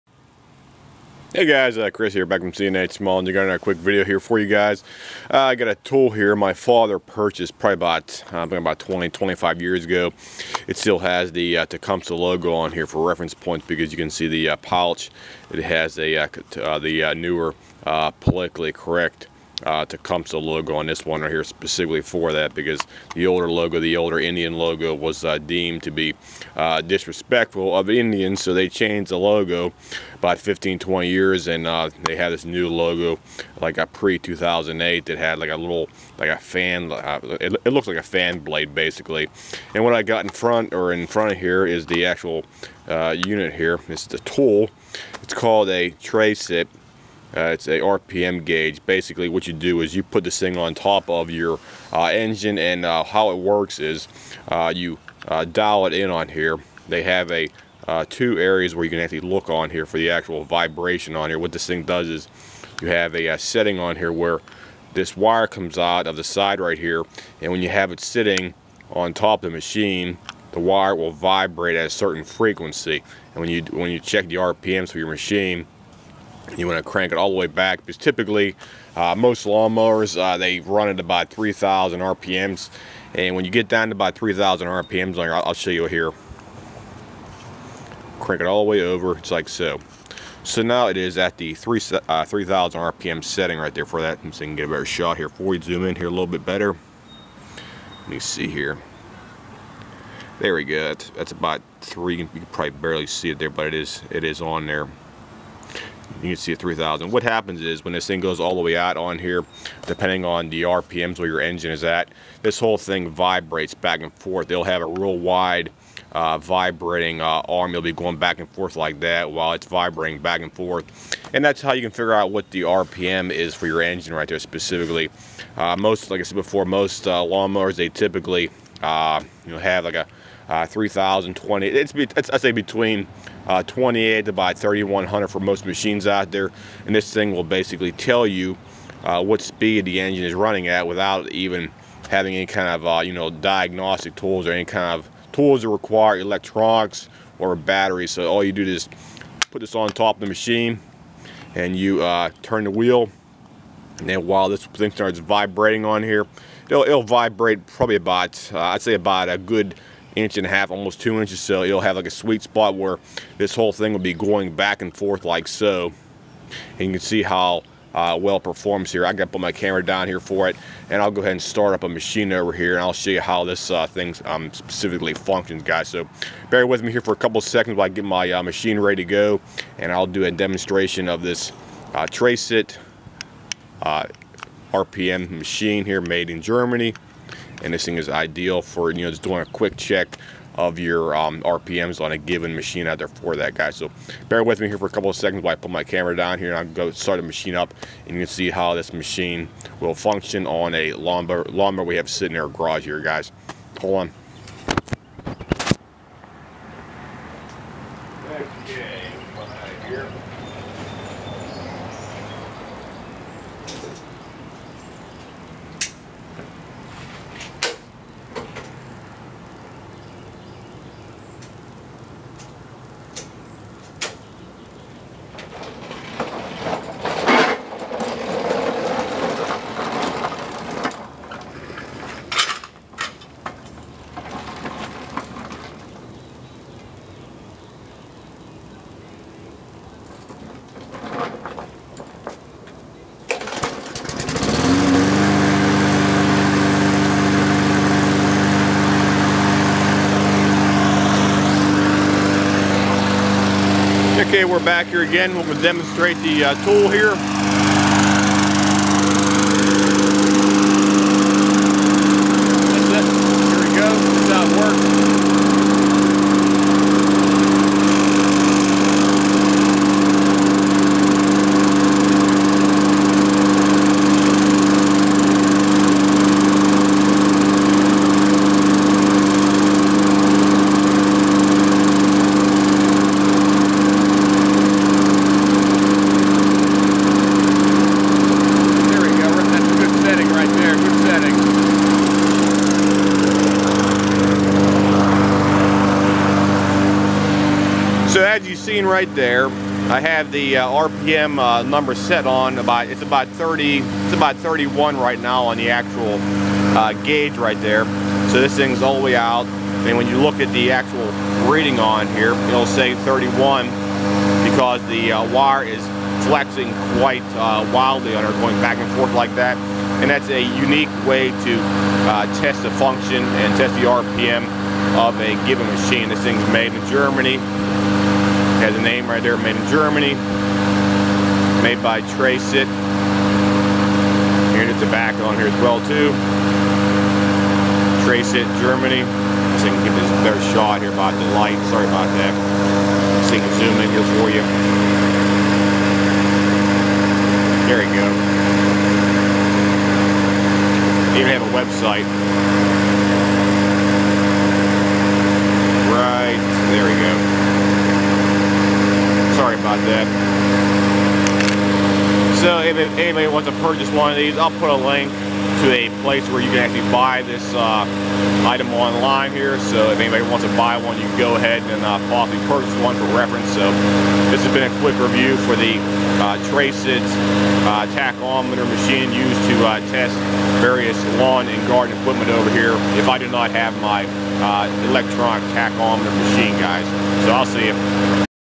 treysit tachometer
i recommend this product for anyone who wants a simple way to verify engine rpm's.